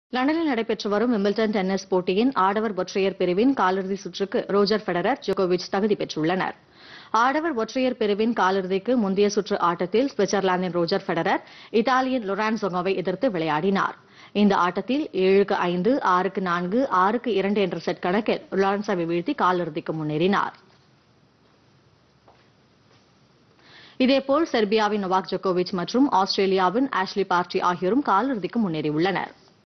காலிறுதியில் ரோஜர் ஃபெடரர், ‍ஜோக்கோவிச்PodhigaiTamilNews
விம்பிள்டன் டென்னிஸ் போட்டி - ரோஜர் ‍ஃ பெடரர், ஜோக்கோவிச் காலிறுதிக்கு தகுதி.